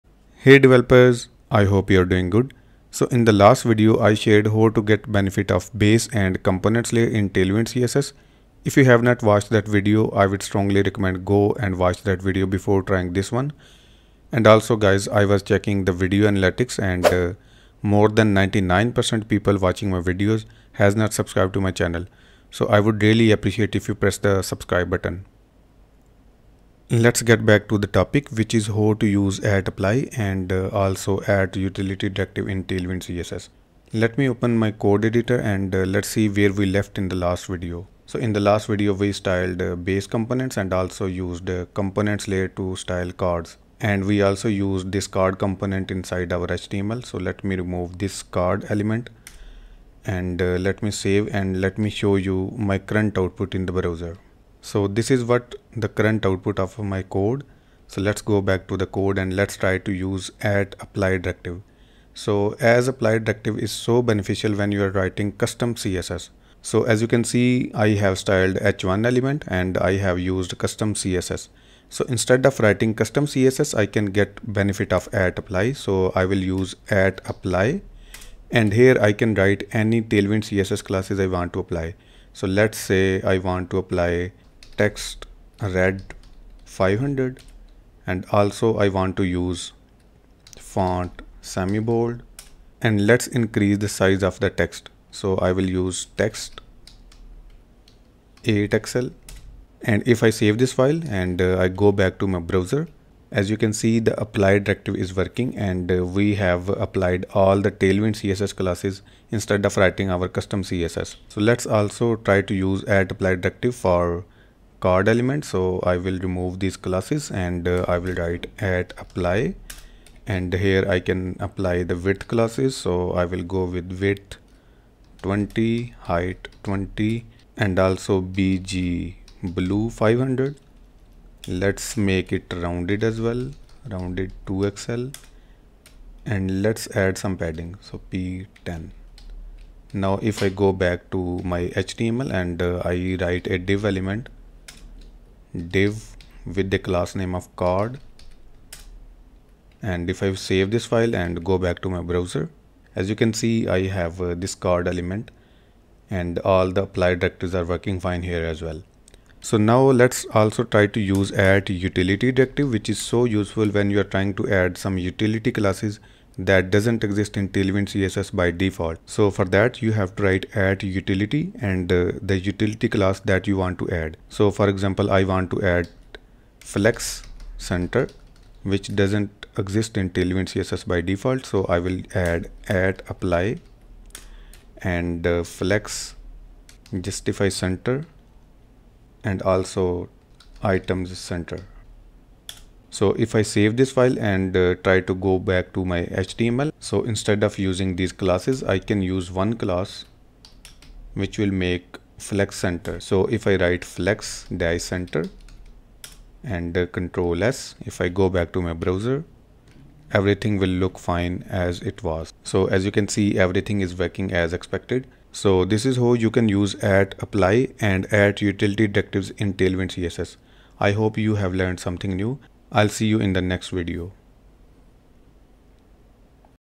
How to use @apply and @utility directives in TailwindCSS v4
In Tailwind CSS, @apply and @utility directives are so beneficial when you want to write custom styles. In this video, I have shared how to use the @apply directive and also the @utility directive.
The first @apply directive will replace all the old styled CSS and will give you option to get benefit of all the TailwindCSS predefined classes.
The second @utiltiy directive is also helpful to add custom utility classes that doesn't exist in TailwindCSS by default. You can write any custom utility you want and also get benefit of @apply directive when using this @utility directive.
If you use these 2 directives in your code, you will be amazed how it helps in custom CSS.